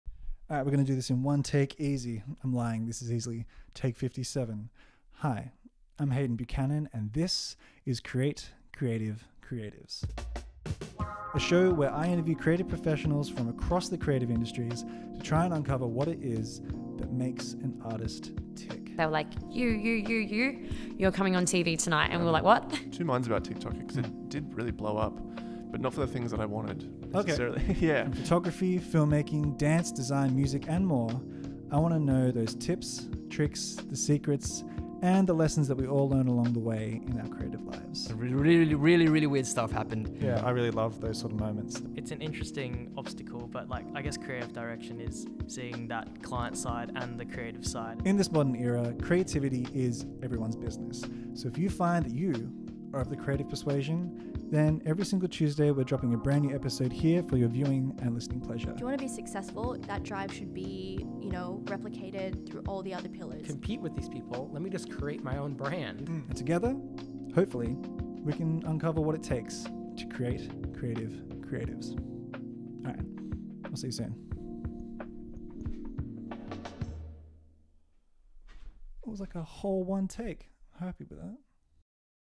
New podcast for creatives - Create Creative Creatives
A new creativity podca

Welcome to Create Creative Creatives my first podcast project where I interview the best creative professionals from across the creative industries to uncover what it is that makes artists tick.

From Photography, Filmmaking, Dance, Design, Music and more I want to uncover those tips, tricks, Secrets and lessons we all learn along the way of our creative lives.

In this day and age creativity is everyone's business

So if you find you, are of the creative persuasion then every Tuesday there will be a brand new episode of the podcast here for your viewing and listening pleasure.
And together we can uncover what it takes to create creative creatives.

Prefer to listen you can find the podcast on all major streaming services.

▬▬▬▬▬ Credits ▬▬▬▬▬
all assets created by Hayden Buchanan.